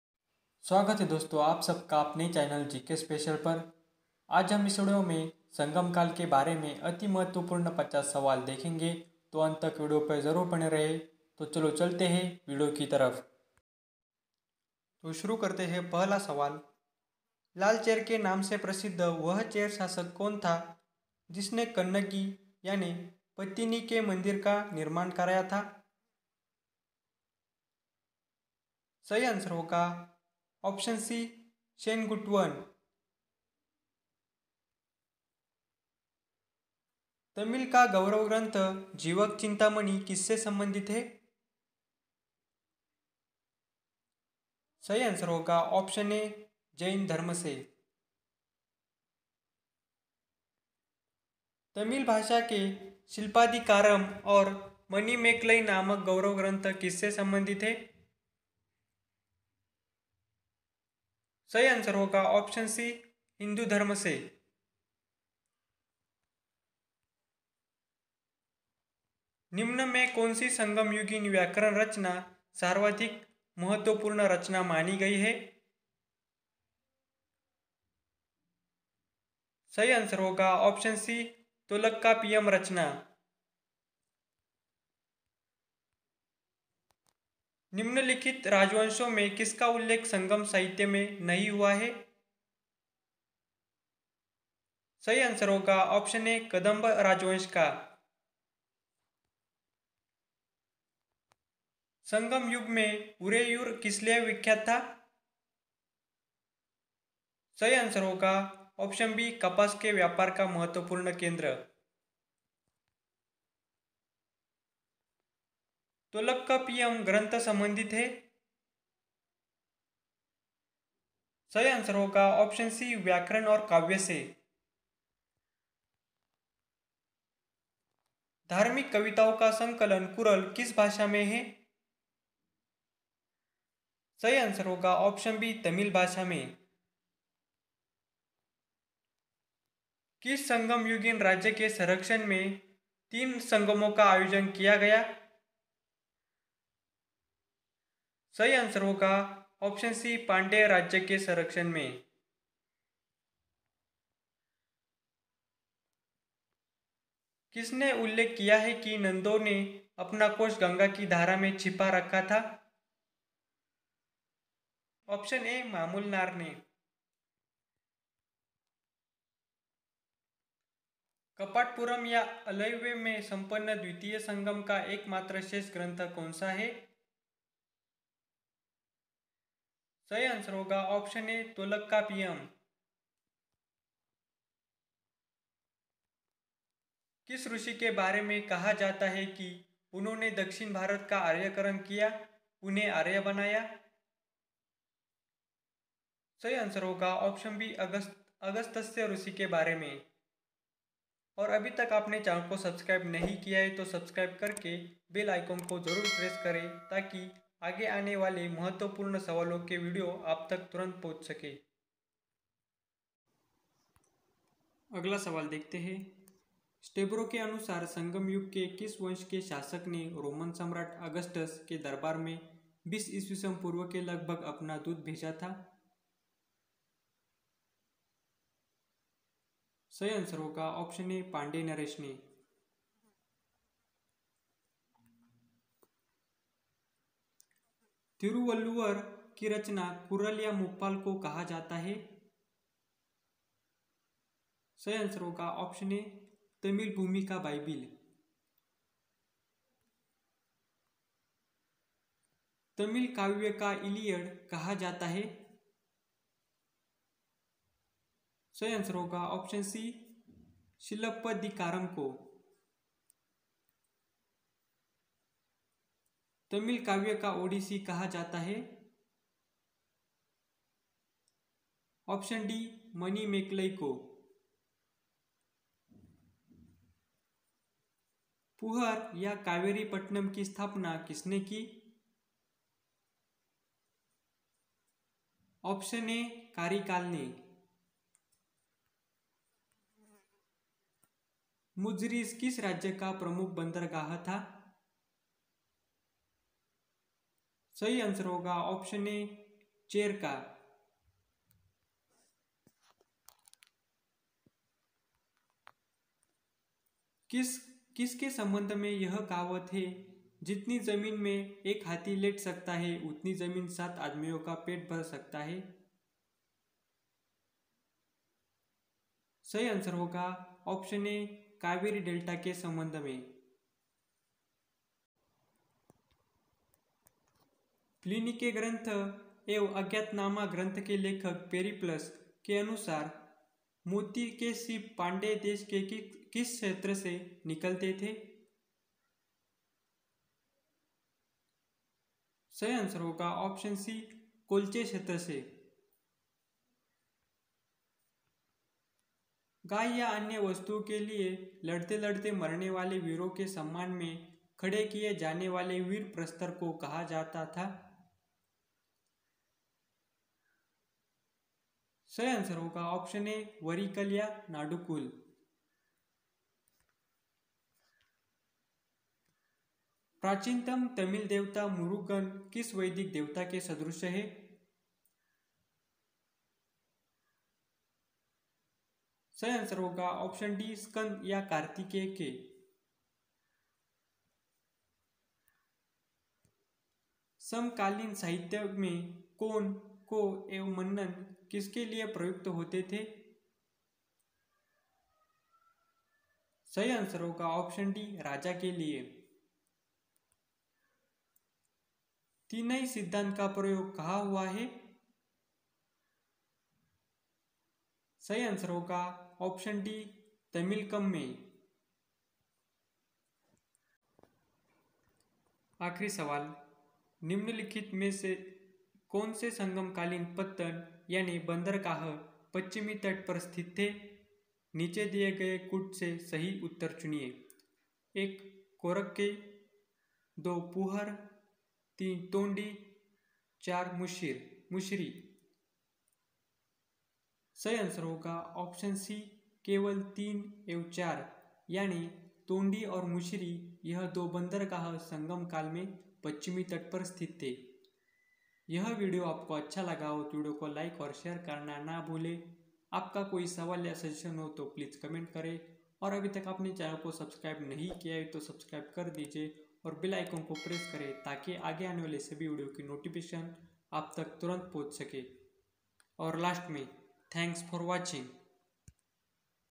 संगम काल || प्राचीन भारत का इतिहास || India history imp 50 gk Questions in Hindi || gk special
gk special
history imp 25 gk Questions in Hindi

इस वीडियो में आपको प्राचीन भारत का इतिहास से संबंधित संगम काल के बारे में महत्वपूर्ण 25 सवाल मिलेंगे तो व्हिडीयो को अंत तक जरूर देखना।